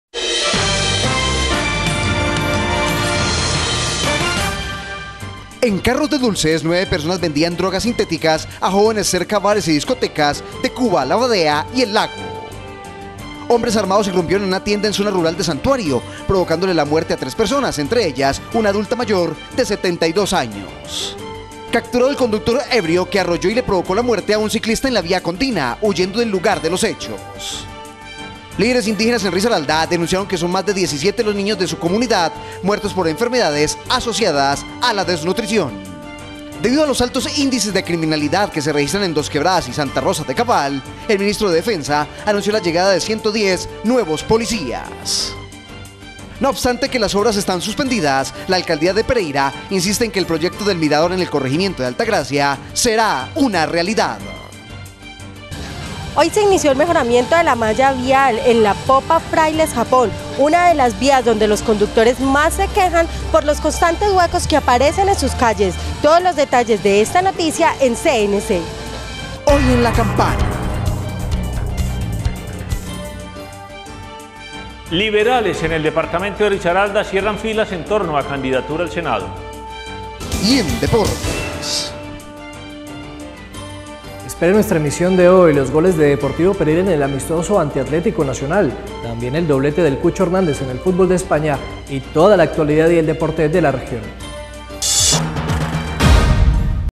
TITULARES CNC NOTICIAS LUNES 09 DE OCTUBRE.
*Chazas con cocaína.
*Triple homicidio en Santuario.
*Lo mató y se volò.
*Dolorosa realidad.
*30 cámaras de seguridad.
*Confirmadas las obras.
*30 huecos tapados.